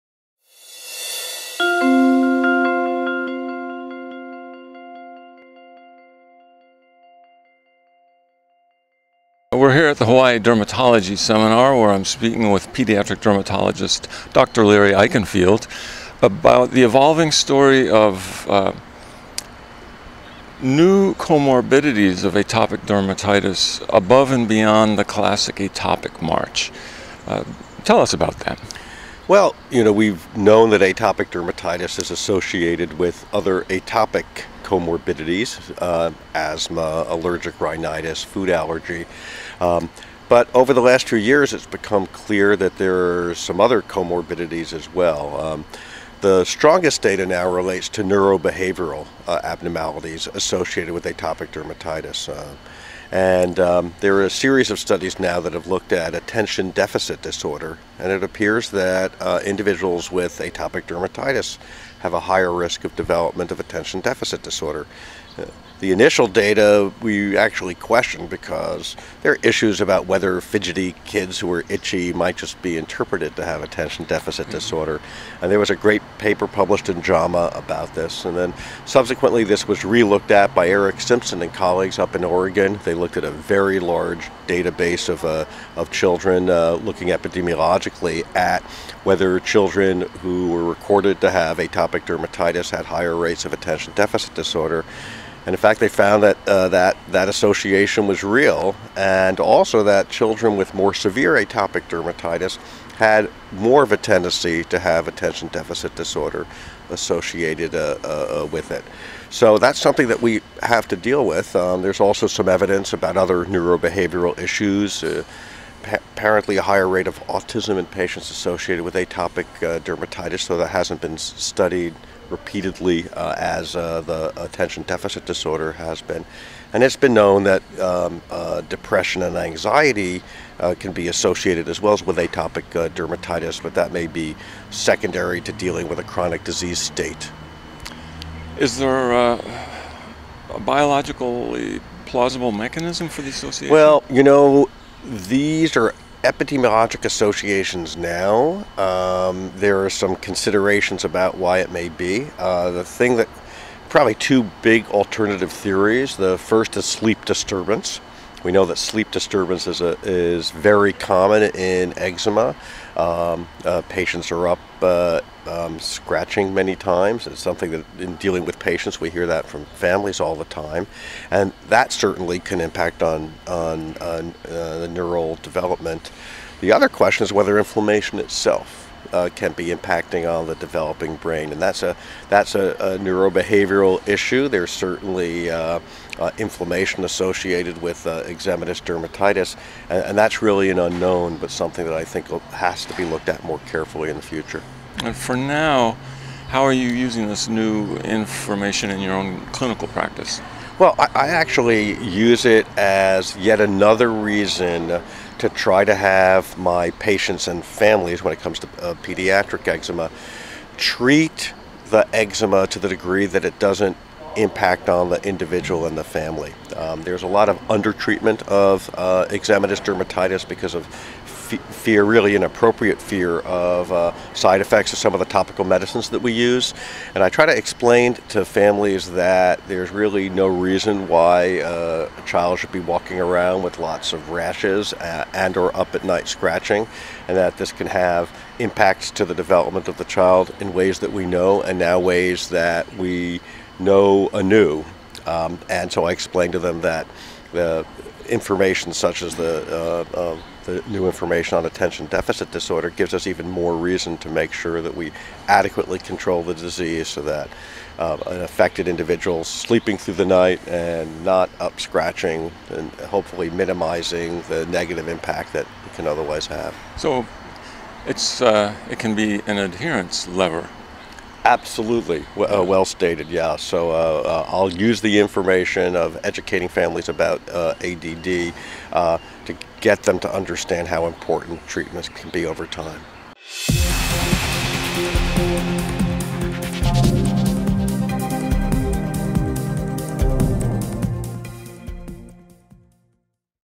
Are atopic dermatitis and ADHD linked?
At the Hawaii Dermatology Seminar, Dr. Lawrence F. Eichenfield of the University of California, San Diego, discusses the evolving perspective on the relationship between atopic dermatitis and attention deficit hyperactivity disorder (ADHD) in children.